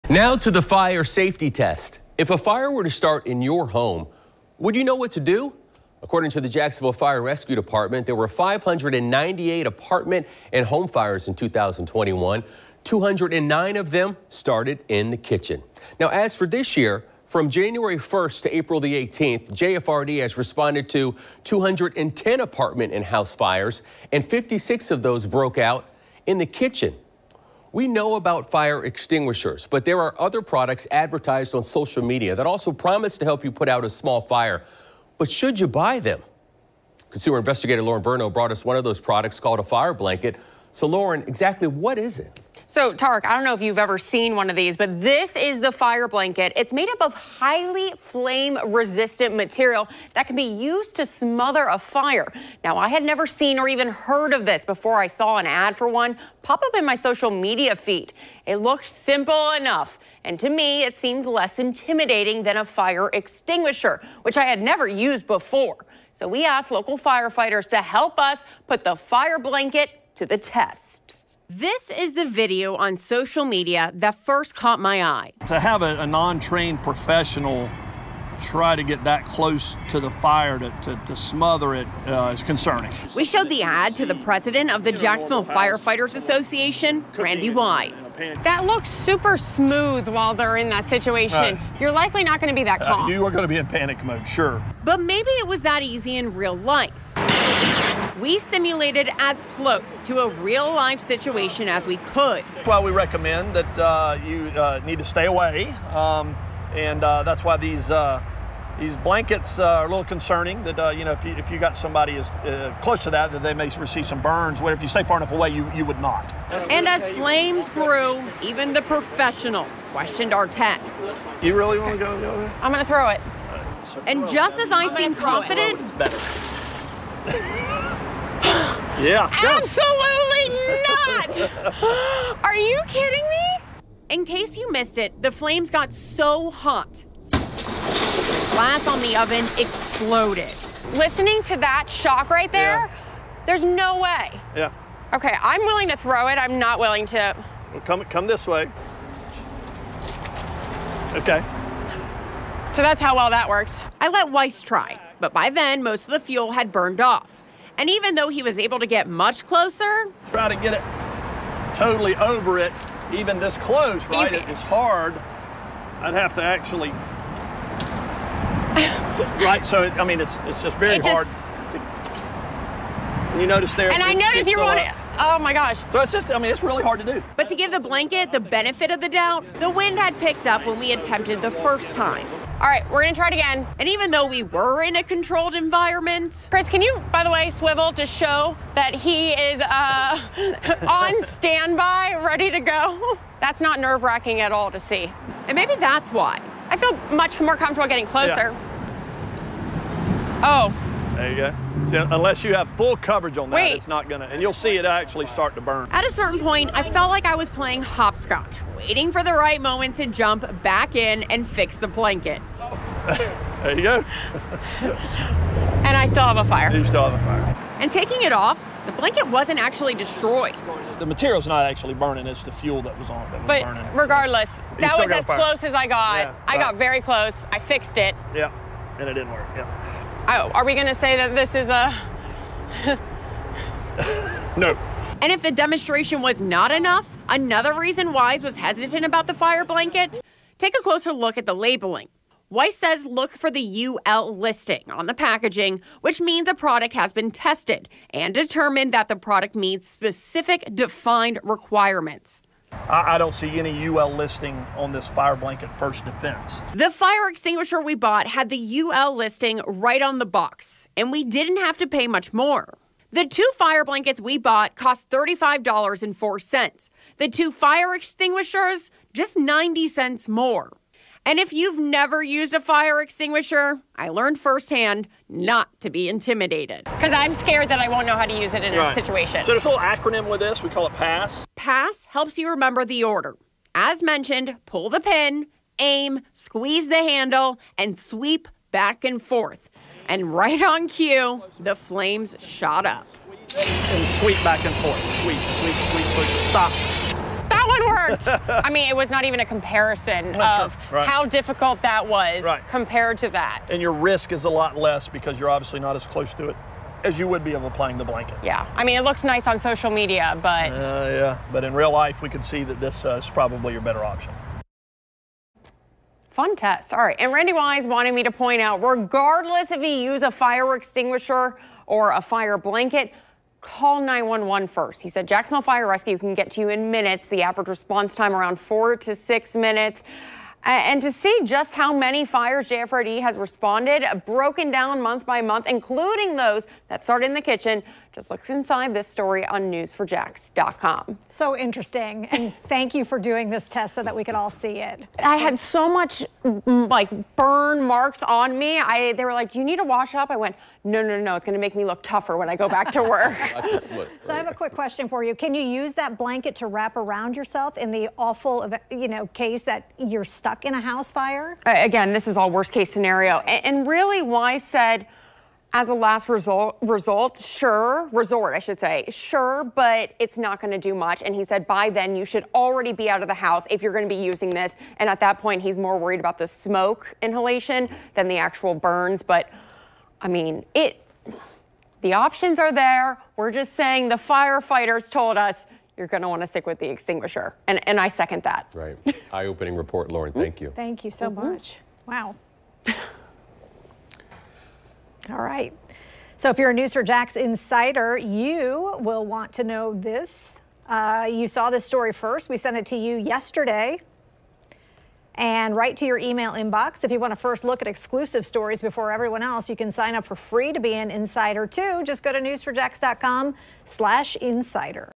Will the fire blanket keep you safe?
We wanted to know if a product promoted on social media really worked.